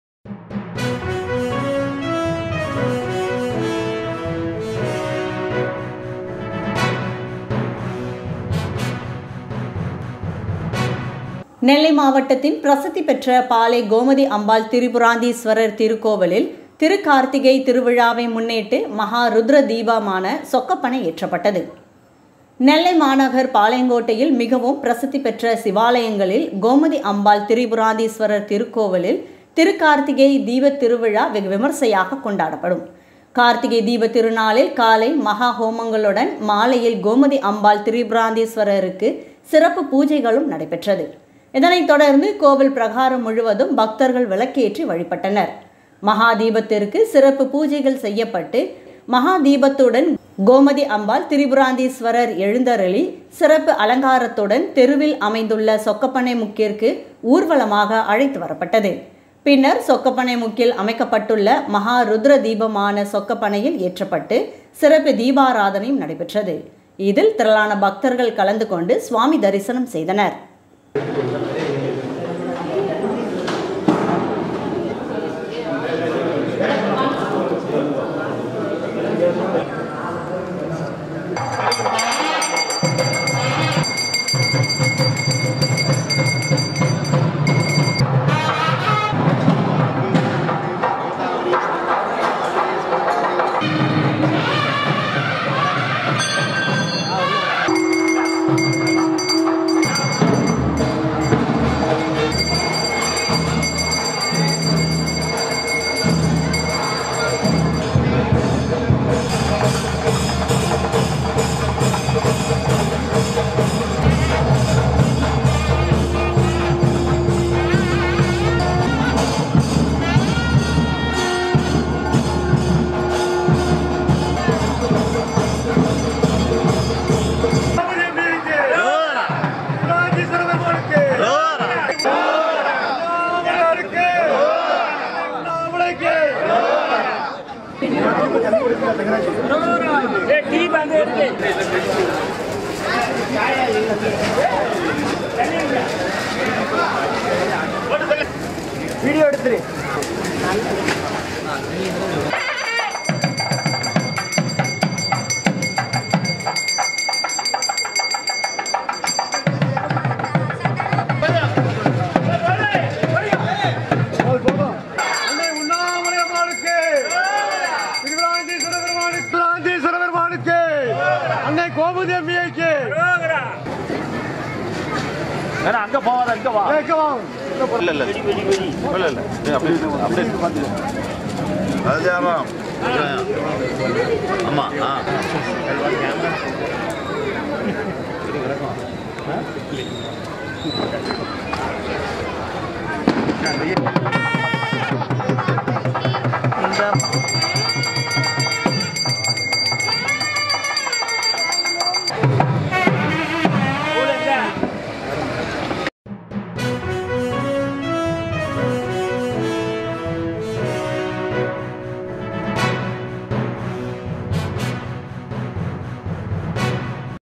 பாளை கோமதி அம்பாள் திரிபுராந்தீஸ்வரர் திருக்கோயிலில் மஹா ருத்ர தீபமான சொக்கப்பனை ஏற்றப்பட்டது.
நெல்லை மாநகர் பாளையங்கோட்டையில் மிகவும் பிரசித்தி பெற்ற சிவாலயங்களில் கோமதி அம்பாள் திரிபுராந்தீஸ்வரர் திருக்கோயிலில் திருக்கார்த்திகை தீப திருவிழா வெகு விமர்சையாக கொண்டாடப்படும். கார்த்திகை தீபத் திருநாளில்  காலை மஹா ஹோமங்களும் மாலையில்  கோமதி அம்பாள் திரிபுராந்தீஸ்வரருக்கு சிறப்பு பூஜைகளும் நடைபெற்றது.  இதனைத்தொடர்ந்து கோவில் பிரகாரம் முழுவதும் பக்தர்கள் விளக்கேற்றி வழிபட்டனர் மஹா தீபத்திற்கு சிறப்பு பூஜைகள் செய்யப்பட்டு  மஹா தீபத்துடன் கோமதி அம்பாள் திரிபுராந்தீஸ்வரர் எழுந்தருளி சிறப்பு அலங்காரத்துடன் தெருவில் அமைந்துள்ள சொக்கப்பனை மூக்கிற்கு ஊர்வலமாக  அழைத்து வரப்பட்டது . பின்னர் சொக்கப்பனை முக்கில் அமைக்கப்பட்டுள்ள மகா ருத்ர தீபமான சொக்கப்பனையில் ஏற்றப்பட்டு  சிறப்பு தீபாராதனையும் நடைபெற்றது .இதில் திரளான பக்தர்கள் கலந்துகொண்டு சுவாமி தரிசனம் செய்தனர்.